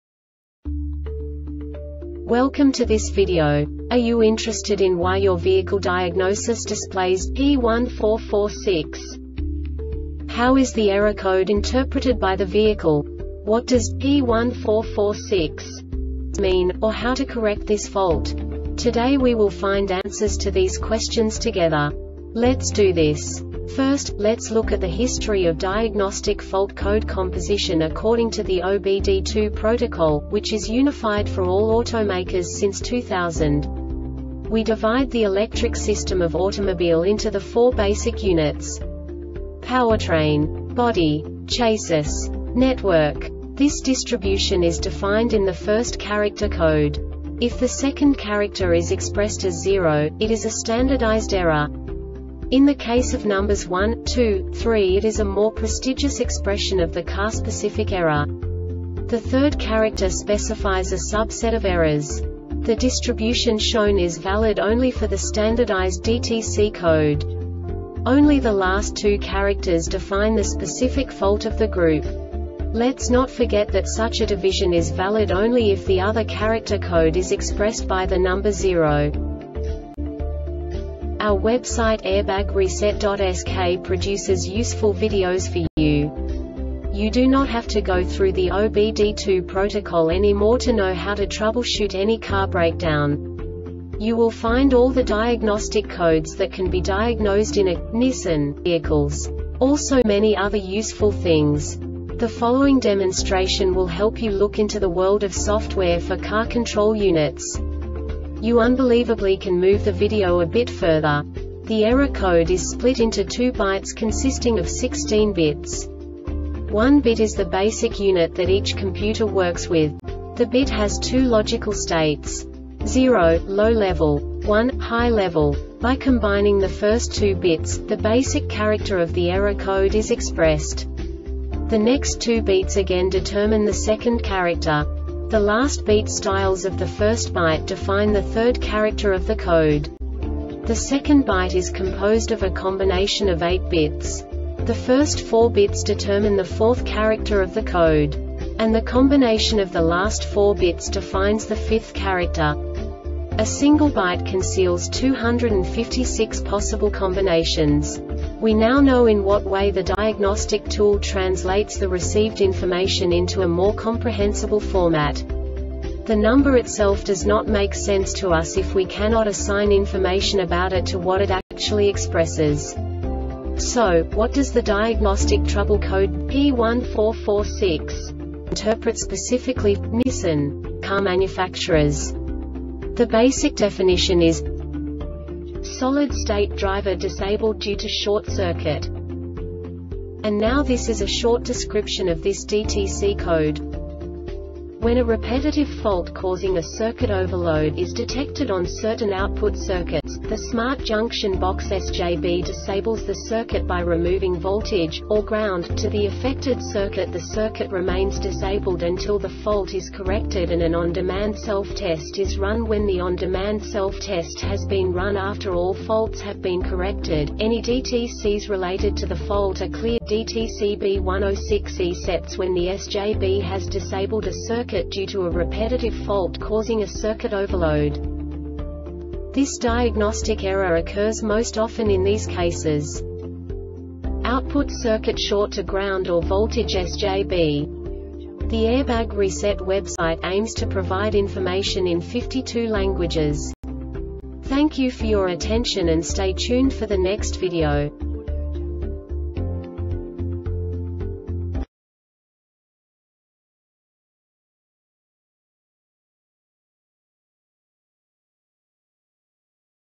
DTC Nissan P1446 Short Explanation
Contents:
0:21 Basic DTC analysis according to OBD2 protocol standard.
1:48 Insight into programming
2:58 A diagnostically understandable description of the Diagnostic Trouble Code
3:05 Basic error definition

Make:
Nissan
Code:
P1446
Definition:
Solid State Driver Disabled Due to Short Circuit
Description:
When a repetitive fault causing a circuit overload is detected on certain output circuits,the Smart Junction Box (SJB) disables the circuit by removing voltage,or ground,to the affected circuit.The circuit remains disabled until the fault is corrected and an on-demand self-test is run.When the on-demand self-test has been run after all faults have been corrected,any DTCs related to the fault are cleared. DTC B106E sets when the SJB has disabled a circuit due to a repetitive fault causing a circuit overload.
Cause:
 Output circuit short to ground or voltage SJB